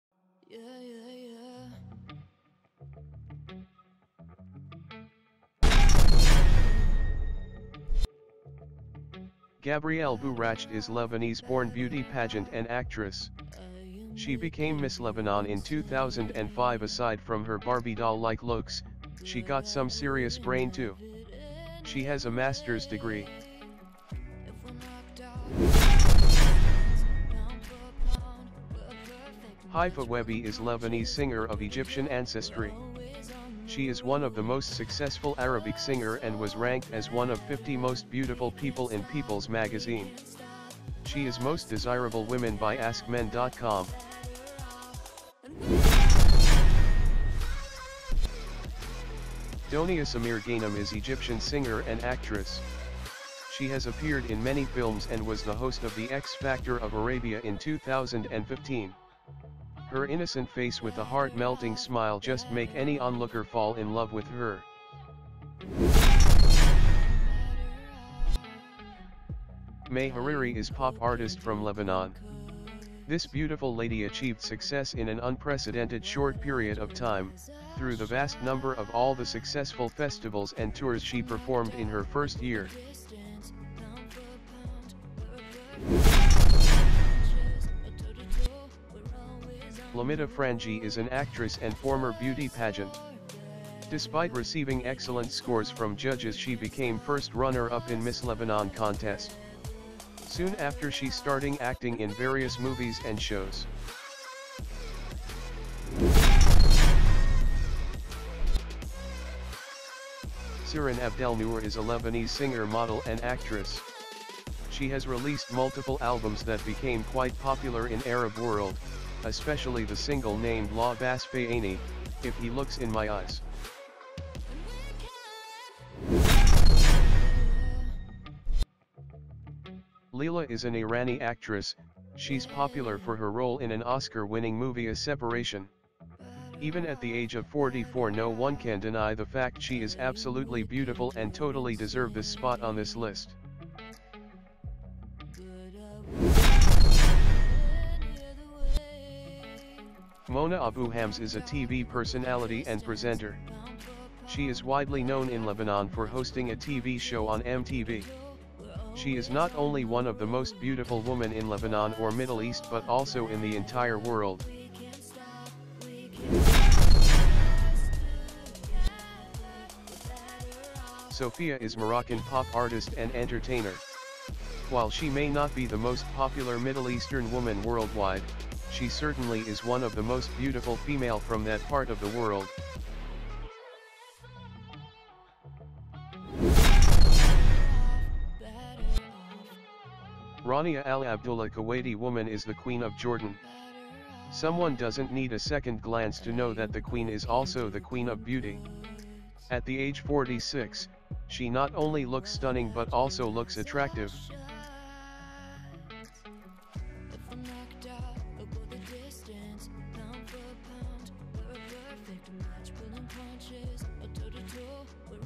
Top 10 Most Beautiful Middle Eastern Women 2021 l Hottest Middle Eastern Women - INFINITE FACTS
Top 10 Most Beautiful Middle Eastern Women 2021 - INFINITE FACTS

We are representing Top 10 things in our channel  INFINITE FACTS, hope you will like our video.
Here are the Top 10 Most Beautiful Middle Eastern Women 2021 GabrielleBouRached HaifaWehbe DoniaSamirGhanem MayHariri LamittaFrangieh CyrineAbdelnour LeilaHatami MonaAbouHamze SofiaElMarikh and RaniaAlAbdullah

Suggested Videos :-